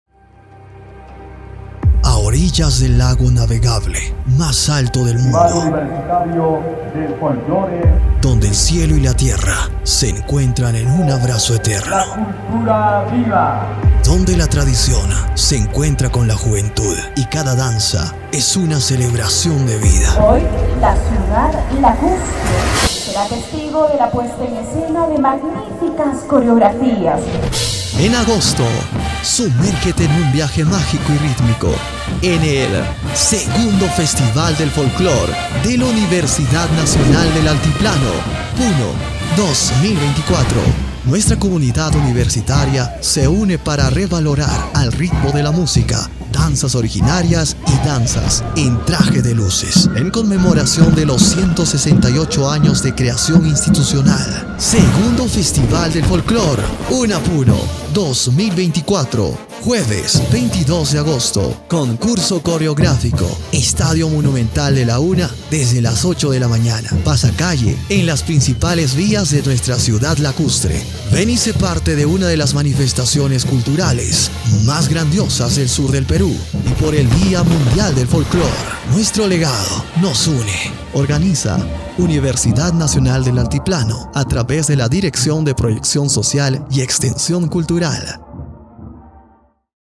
🎉 "Festival Universitario del Folklore 2024"
🎉 ¡Una fiesta de cultura, danza y música! La Universidad Nacional del Altiplano Puno presenta el "Festival Universitario del Folklore 2024", Concurso de Danzas Originarias y Concurso de Trajes Especiales.

📌 Estadio Monumental UNA Puno.

¡Puno es cultura!💙